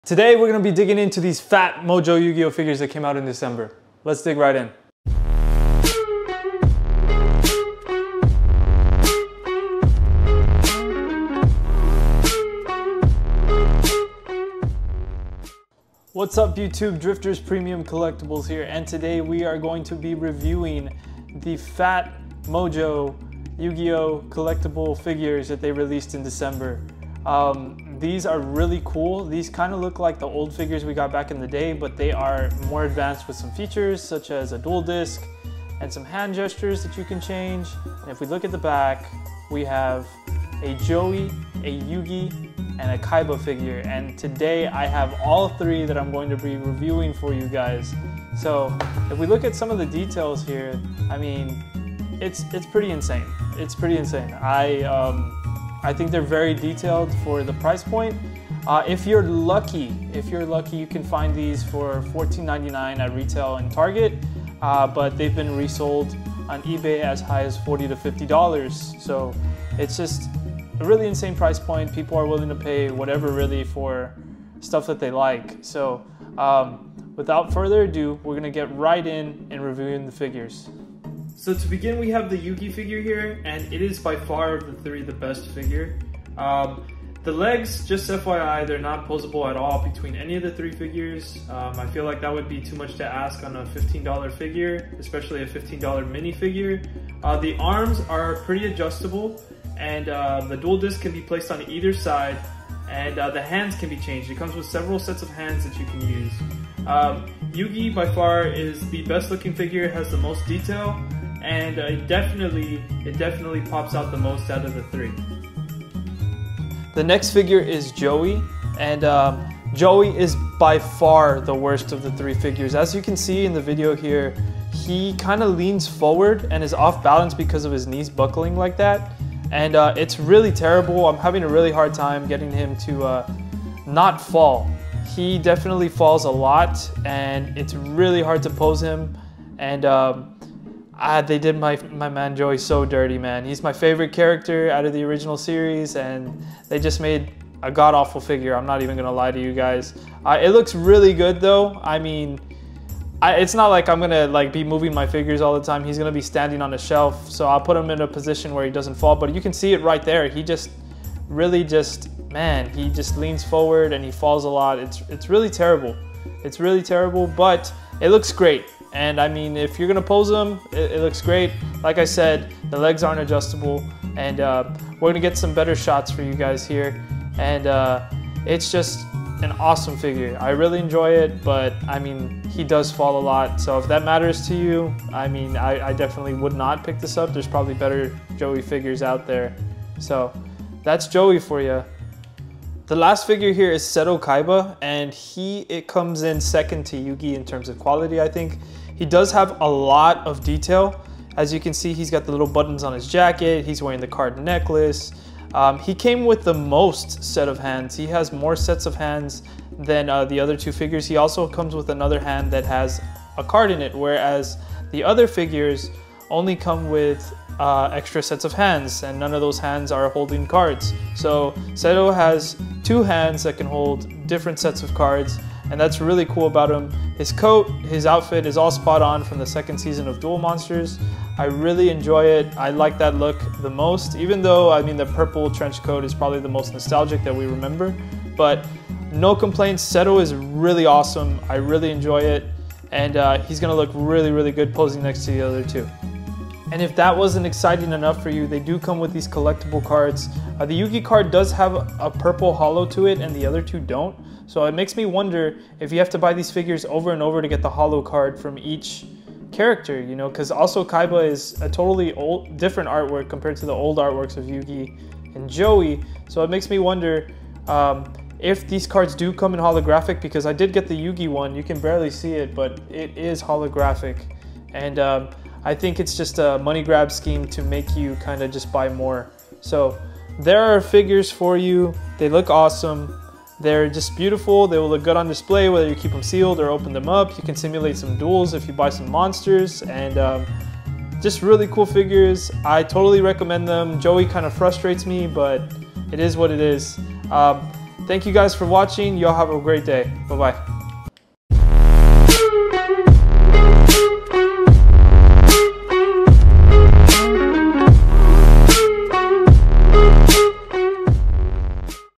Reviewing PhatMojo’s 2022 Yugioh Figures
Today we are reviewing the 2022 PhatMojo Yugioh Figures!